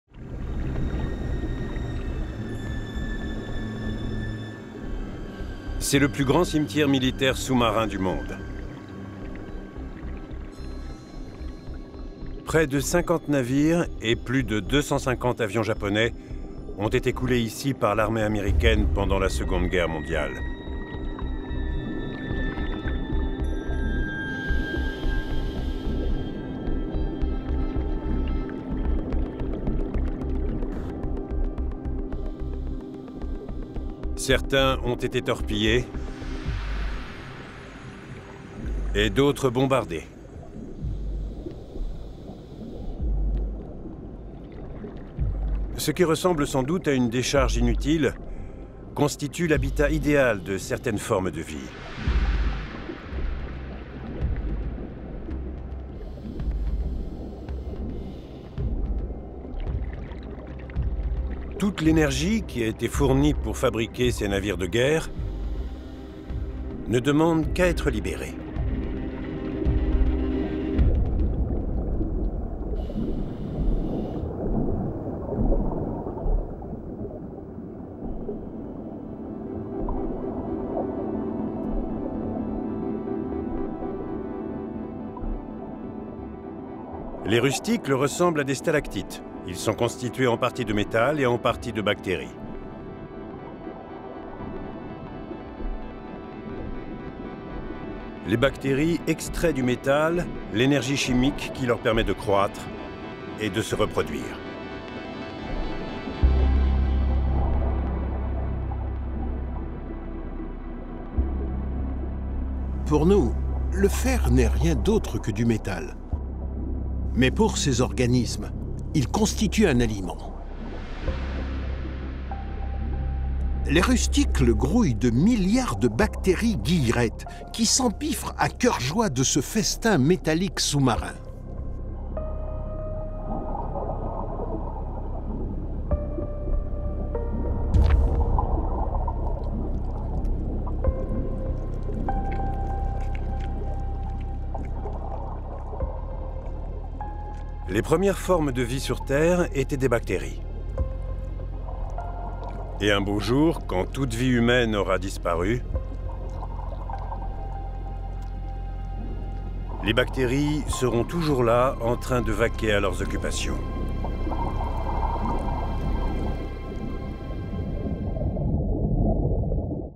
Découvrez le plus grand cimetière militaire sous-marin du monde
Près de 50 navires et plus 250 avions japonais ont été coulés ici par l’armée américaine pendant la Seconde Guerre mondiale. Toute l’énergie qui a été fournie pour fabriquer ces navires de guerre ne demande qu’à être libérée…

National Geographic, disponible avec Canal +, canal 115.